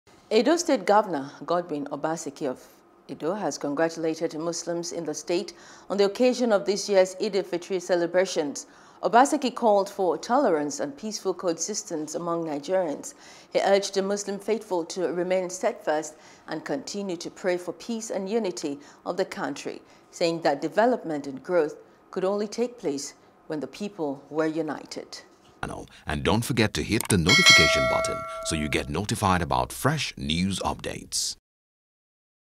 EID EL-FITR: Obaseki Preaches Tolerance, Peaceful Coexistence Among Nigerians | NEWS
Edo State Governor, Godwin Obaseki of Edo has congratulated Muslims in the state on the occasion of this year’s Eid-el Fitr celebrations.